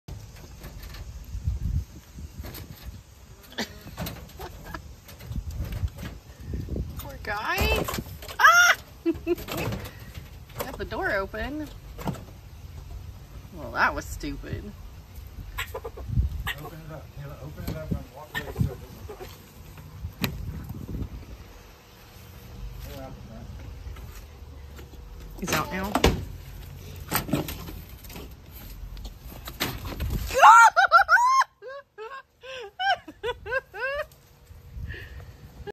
Pet squirrel gets out of cage
A squirrel gets caught in a small cage. After several attempt it gets free and runs away.